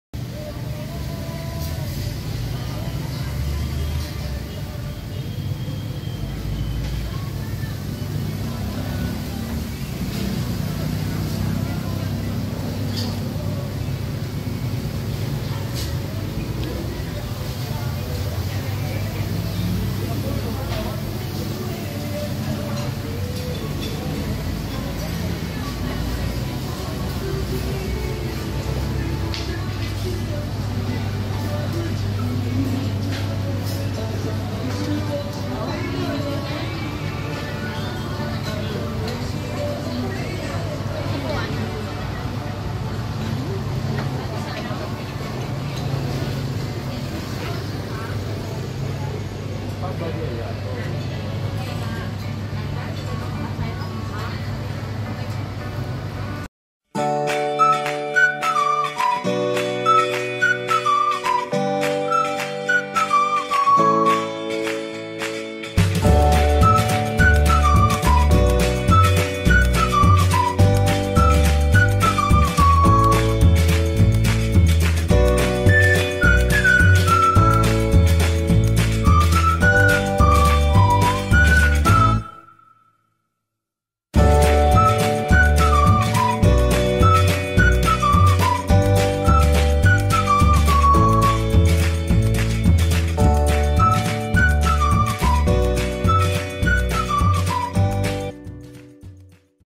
SABAI SABAI GARDEN Restaurant At Jalan Ong Yi How, B'worth Offers Excellent Authentic Thai Seafood.
Sabai Sabai Garden Restaurant offers authentic Thai seafood cooked or fried you can find in the menu. There are plenty of dishes in the store that you can choose from.

Location :
4601, Jalan Ong Yi How,
Kampung Simpah,
Butterworth,
Penang.
Operation hours : 11.00am to 3.00pm and then 4.30pm to 11.00pm. Daily

Hope you enjoyed the video. I’ll be bringing you more interesting and exciting videos EVERYDAY.